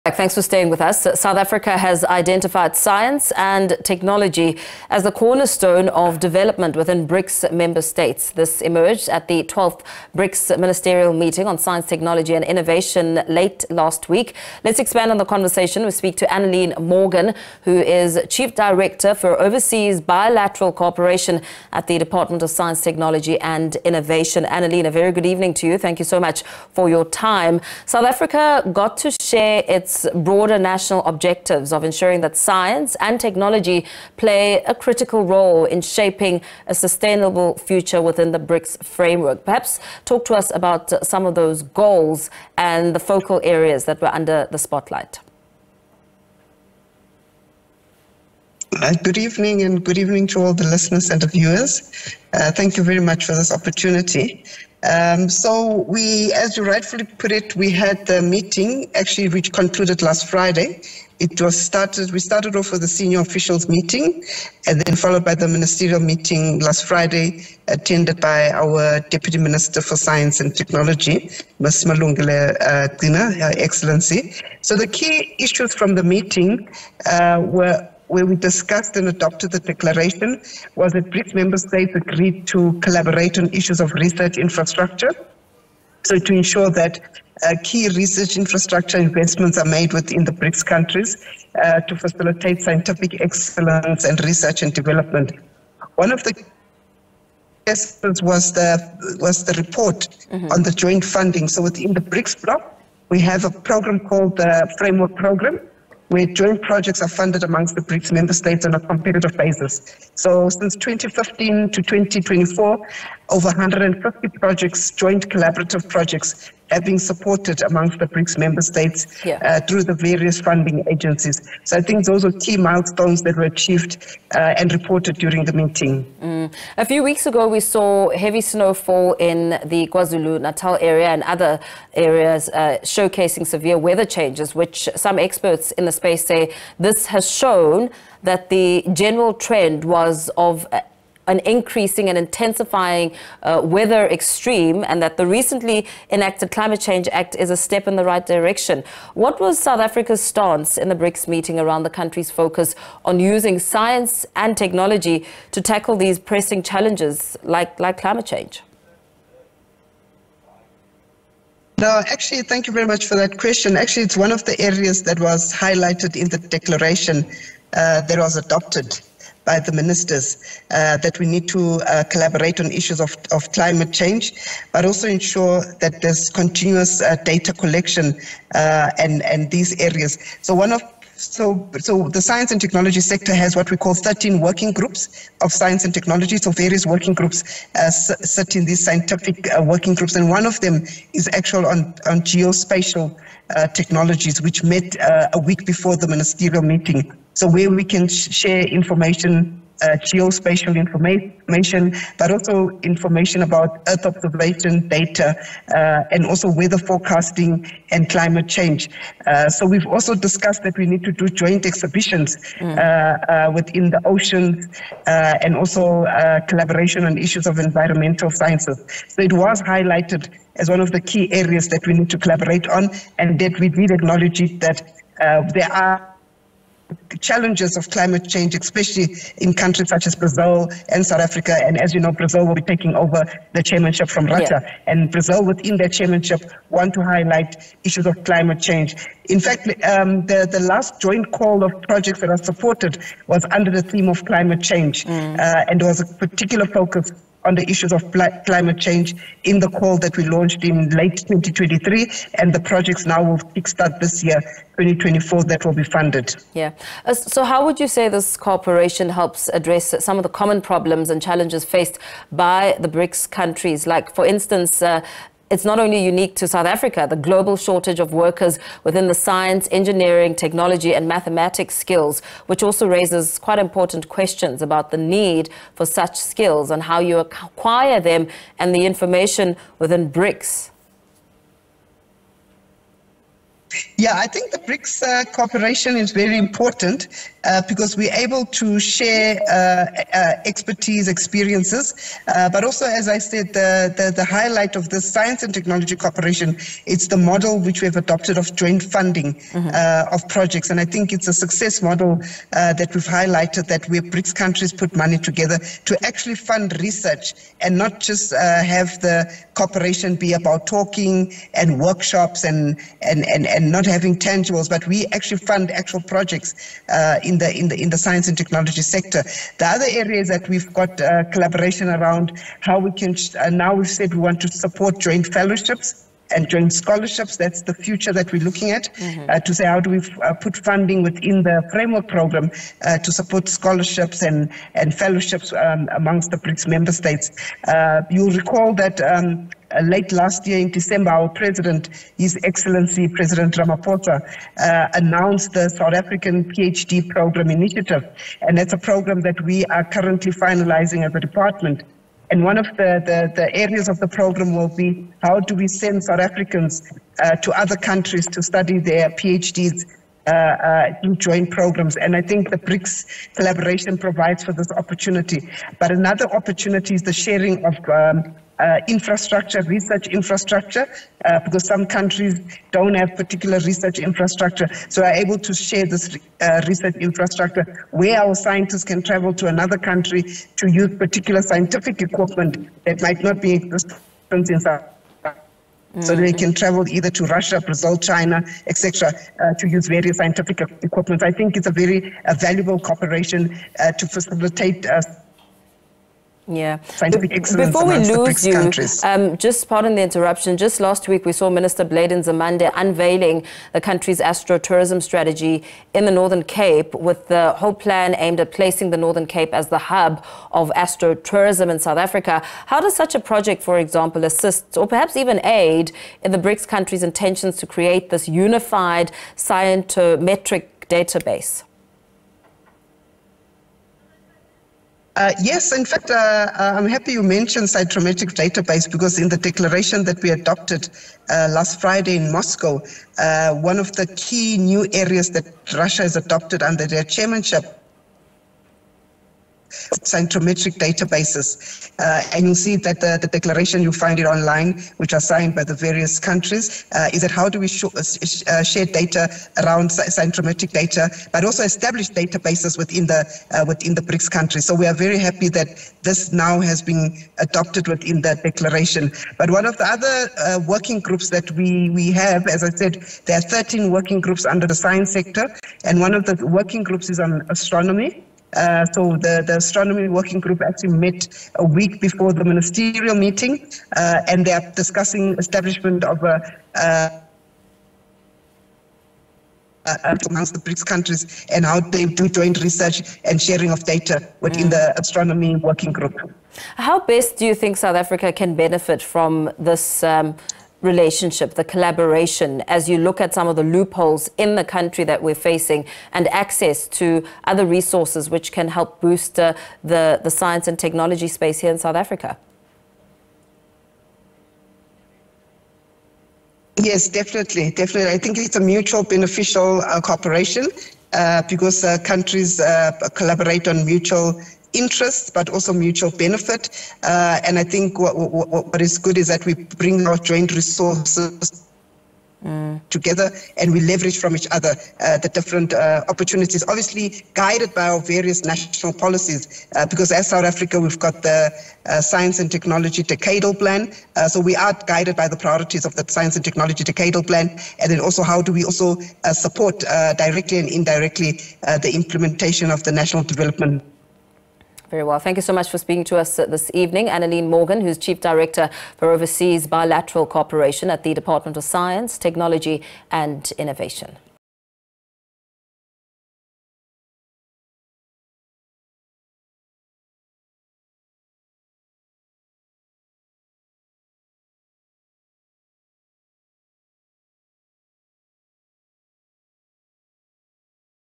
BRICS Ministerial Meeting | Weather Forecasting & Climate Change: Anneline Morgan
South Africa has identified Science and Technology as the cornerstone of development within BRICS member states. This emerged at the 12th BRICS Ministerial Meeting on Science, Technology and Innovation late last week.